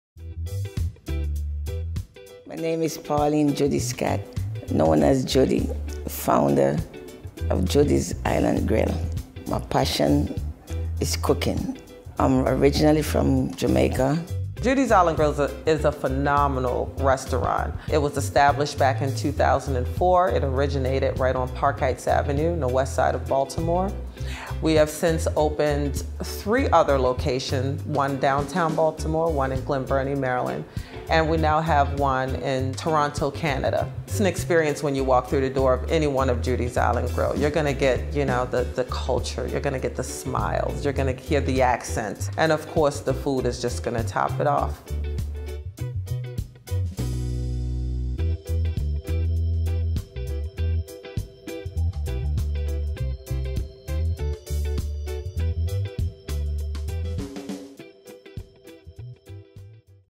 Judy's Island Grill
Judy’s Island Grill is a family owned and operated business originally from Clarendon, Jamaica. Judy’s Island Grill (JIG) has 2 locations within the DMV area! JIG consistently delivers the highest quality, best tasting Caribbean dishes which include Jerk chicken that is to die for! JIG takes pride in hosting events that people from any background or ethnicity can enjoy! JIG is bringing their amazing flavor and upbeat Caribbean spirit to YOU with the Judy’s Carabana Food & Music Festival. We invite you to partake in delicious island drinks, live music, arts/cultural displays, and plenty of Caribbean dishes... including Judy’s jerk chicken!!